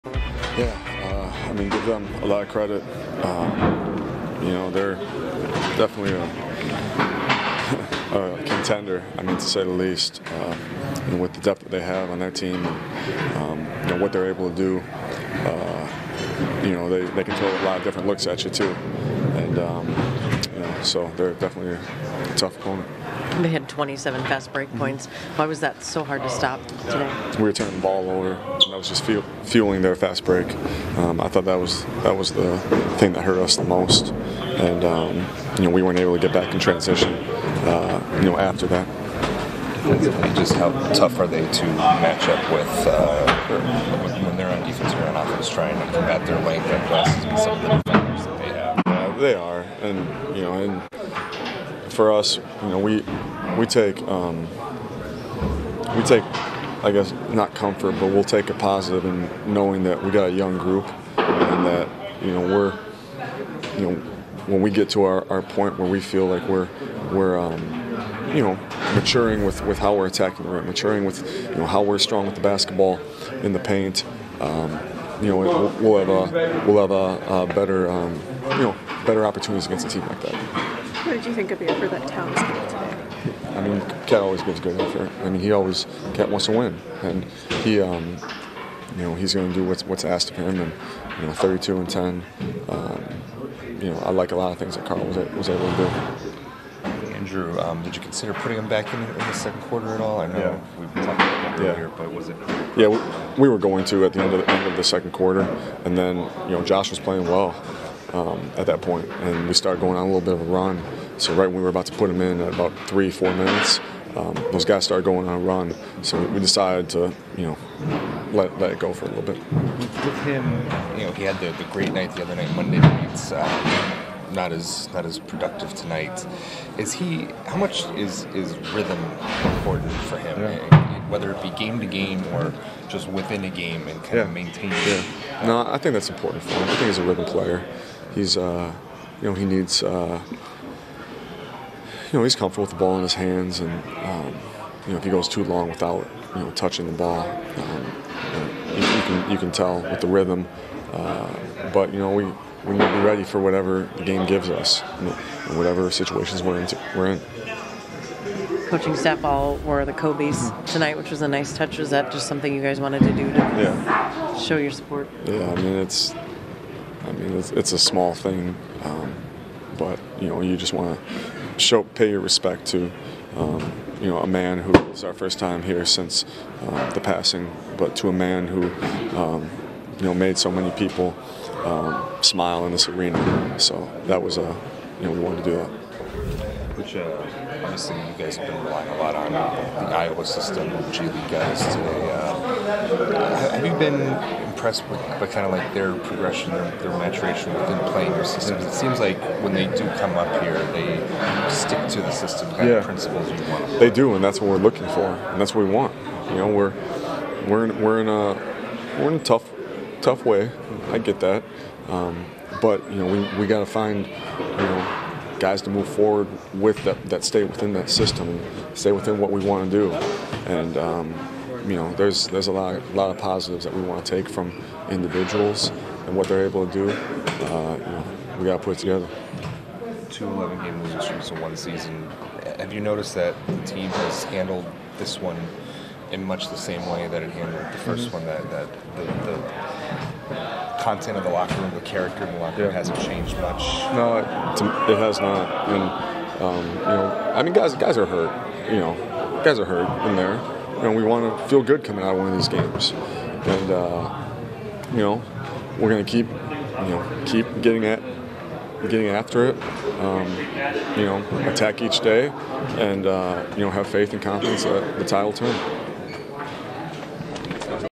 Ryan Saunders on Timberwolves' turnover woes in Los Angeles
Head coach Ryan Saunders speaks on the Minnesota Timberwolves turnover woes and breaks down the 118-106 loss to Los Angeles.